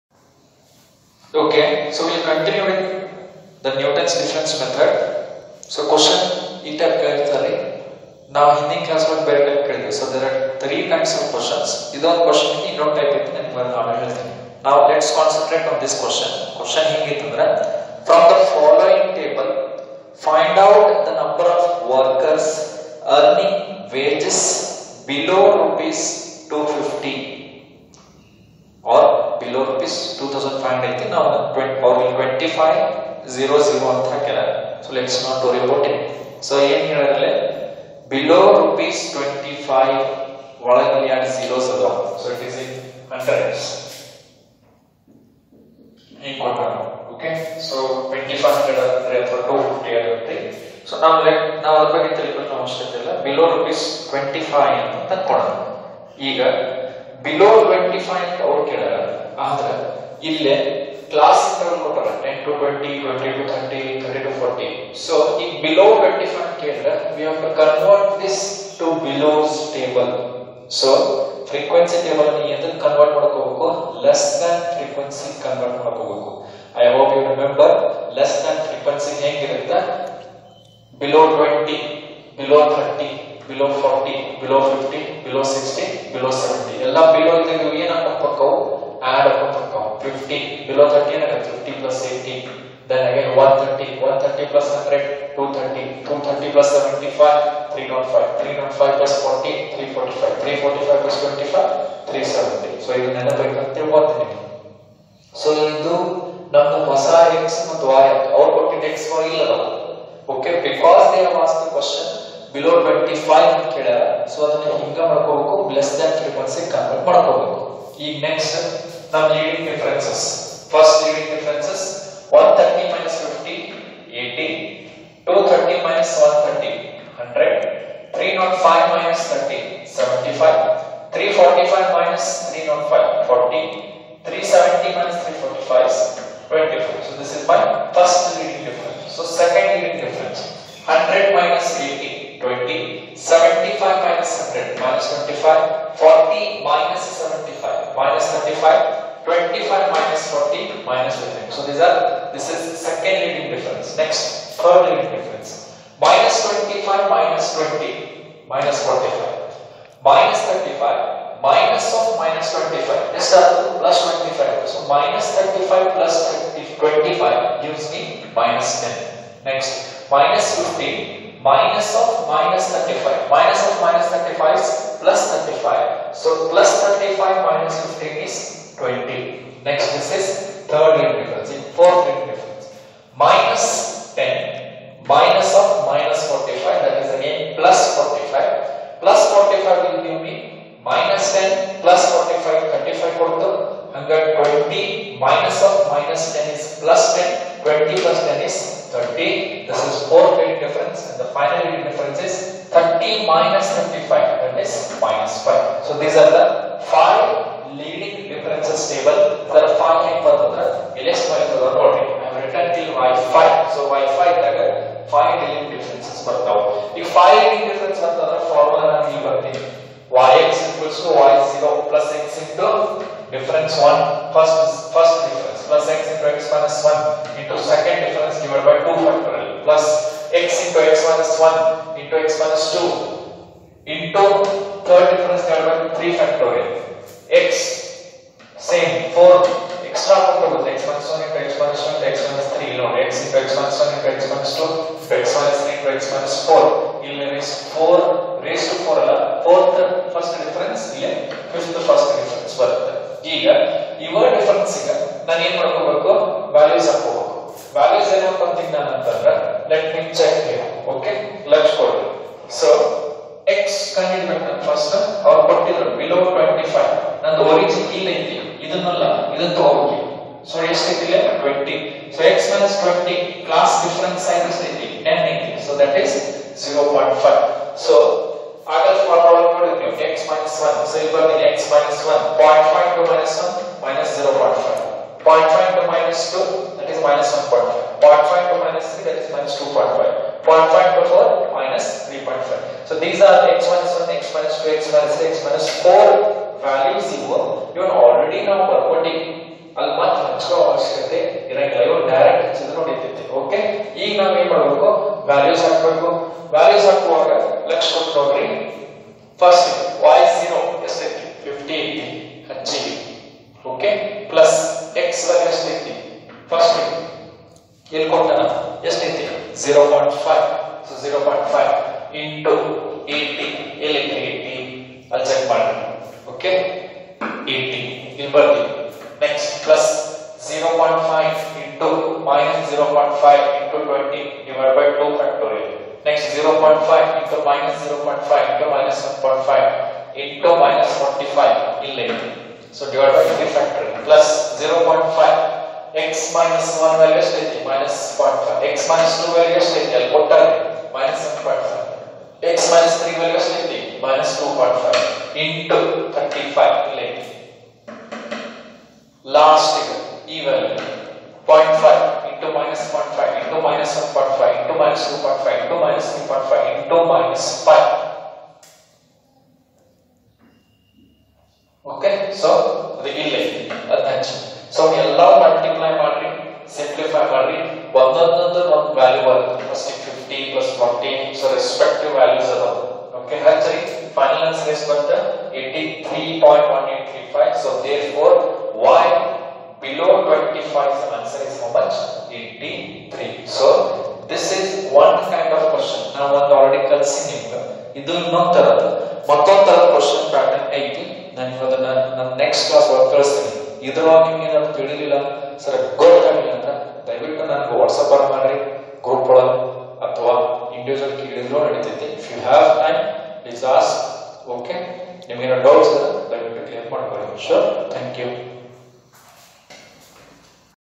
Sanmarg Pu college /2nd Puc Statistics lesson 4 Interpolation and extrapolation part 4(2nd upload)
2nd Puc Statistics lesson 4 Interpolation and extrapolation part 4(2nd upload)